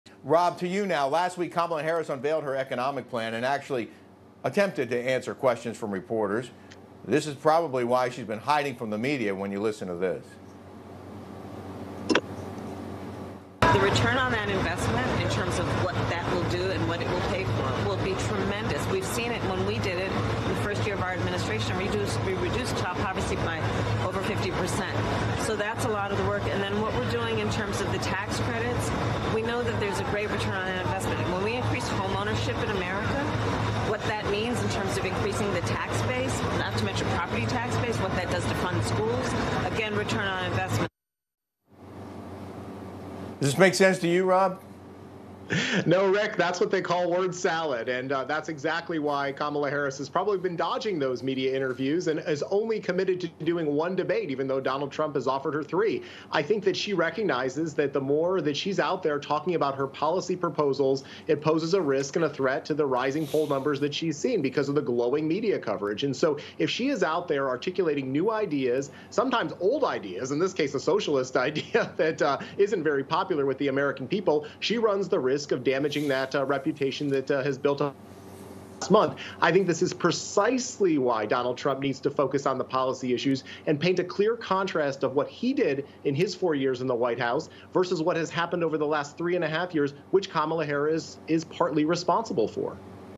Kamala Dodges Media, but Can't Hide Her Socialist Agenda
Newsmax host Rick Leventhal interviews The Daily Signal's Rob Bluey about Vice President Kamala Harris' recently unveiled economic plan and the potential risks it poses to her rising popularity.

Is Harris avoiding tough media questions, and why has she committed to only one debate, despite Donald Trump's offering three?

Bluey explains how her socialist-inspired policies could impact her reputation and the implications for the 2024 election. Plus, he examines how these proposals compare to Trump's record during his four years in the White House.